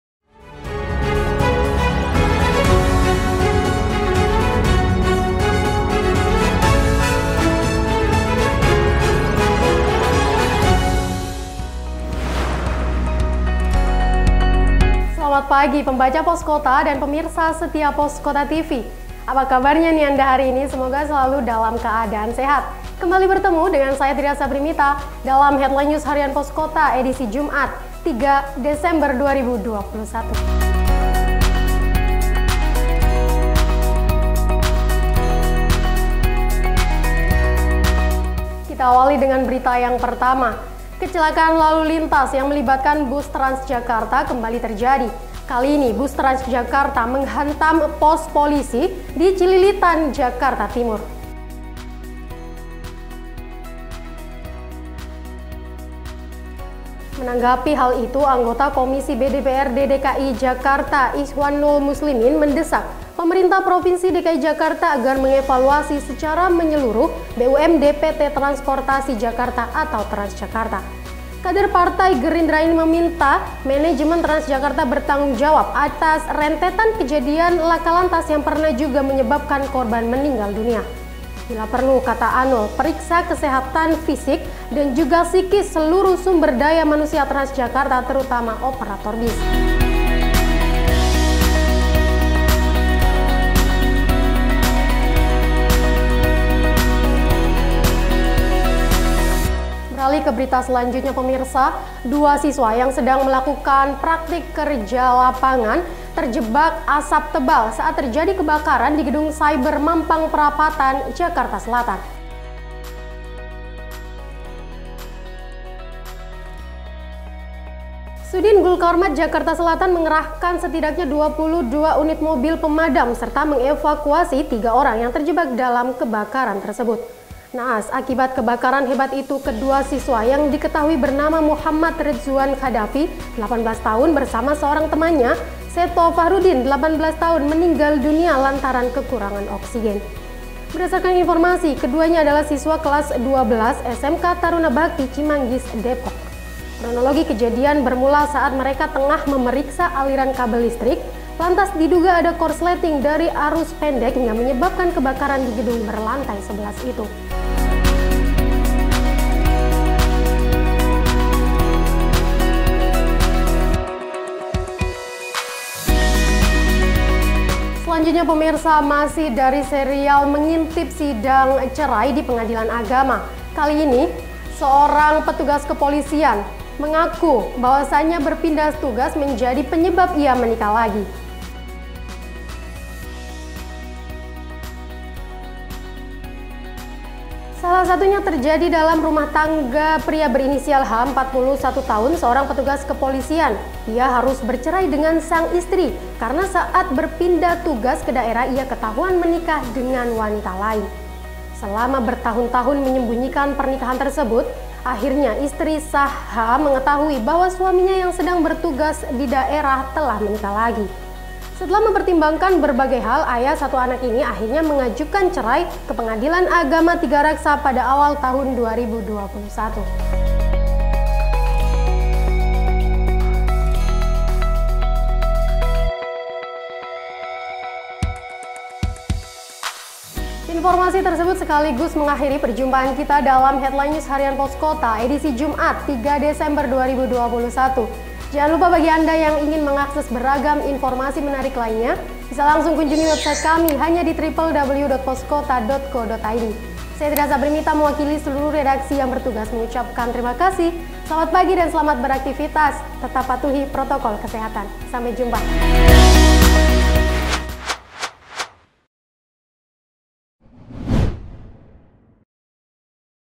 HEADLINE HARIAN POSKOTA EDISI JUMAT 3 DESEMBER 2021
Harian Poskota menyajikan beberapa topik menarik di antaranya bus TransJakarta menghantam pos polisi di Cililitan, kebakaran gedung Cyber tewaskan 2 siswa PKL, serial mengintip sidang cerai di Pengadilan Agama